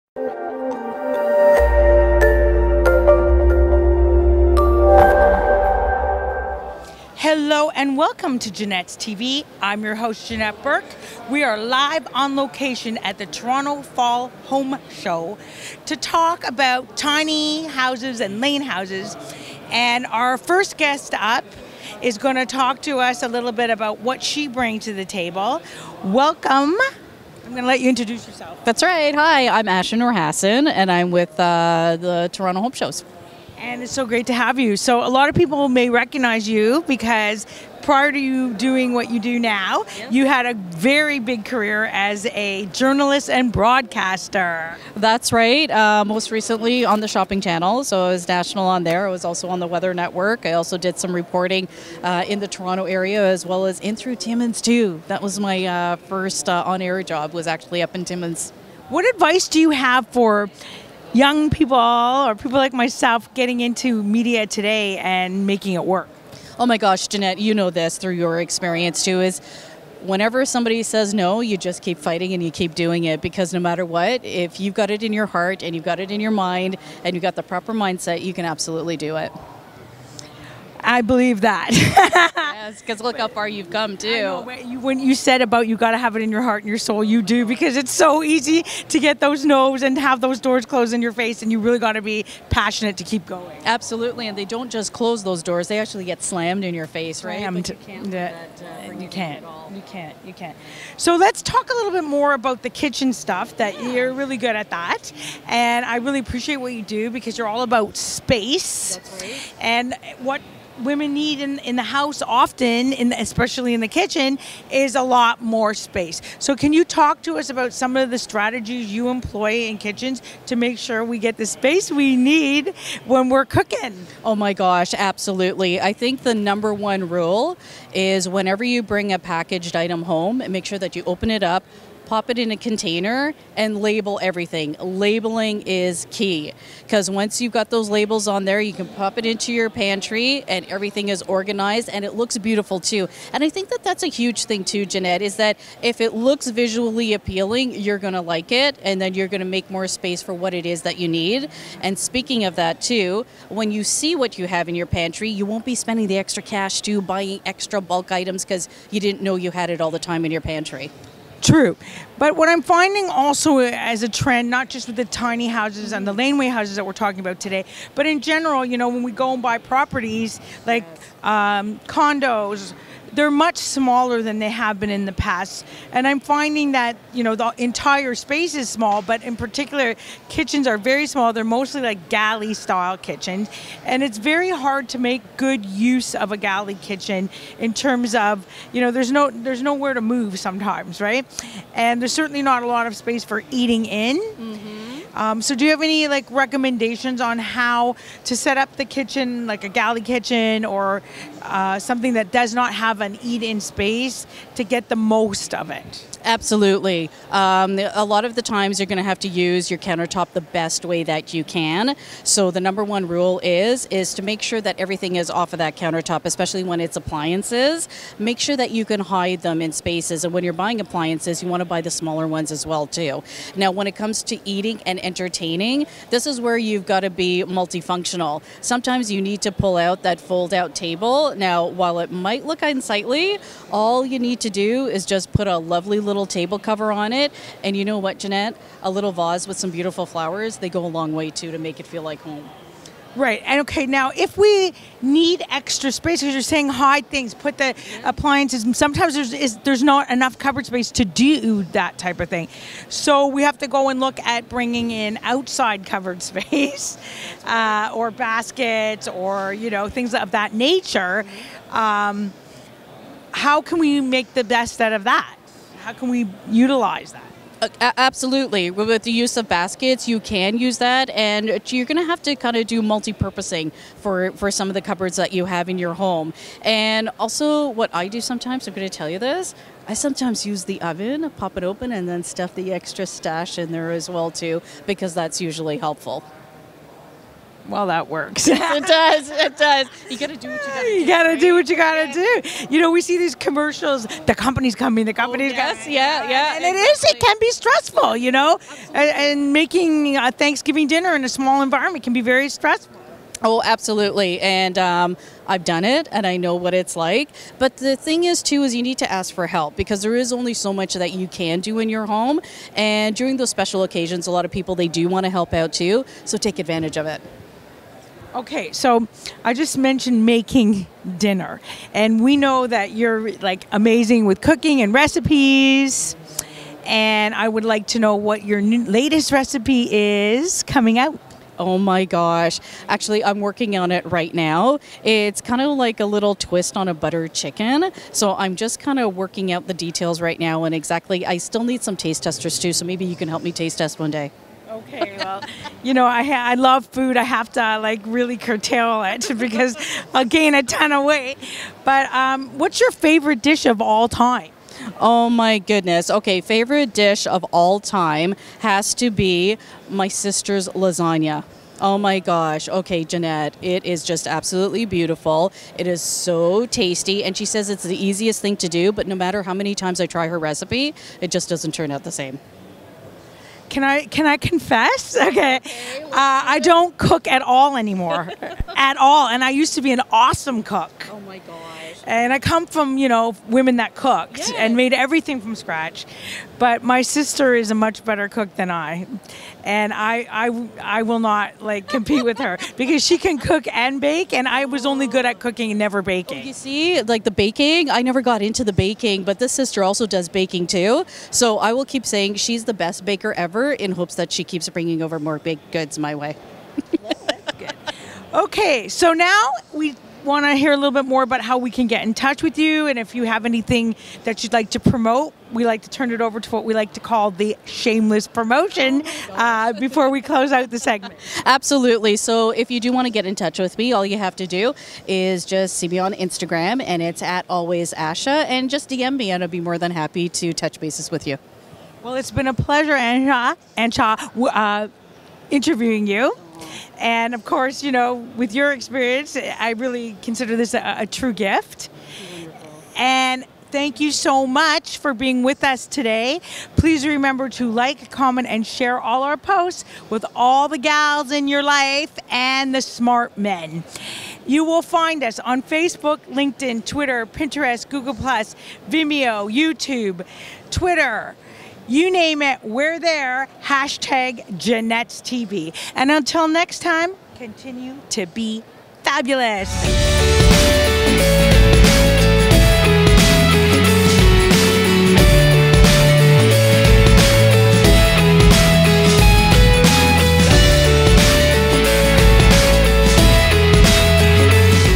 Spruce-up Your Home with Ashaw Noorhasan’s Design & Space-Saving Advice
New Janette’s TV episode – Need some design and space-saving ideas to spruce up your home this year? In this Janette’s TV episode, Janette catches up with Canadian Journalist and former Shopping Channel host, Ashaw Noorhasan at The Toronto Fall Home Show. Please leave your comments in the post and share us with all the amazing women and smart men in your lives. And please hit the BELL to be notified of all our upcoming episodes.

Filming & Editing – Dusko Jocic

Show Graphics – Danielle Caruso

Wardrobe, Hair, Makeup, Janette Burke

Filmed live-on-location at The Toronto Fall Home Show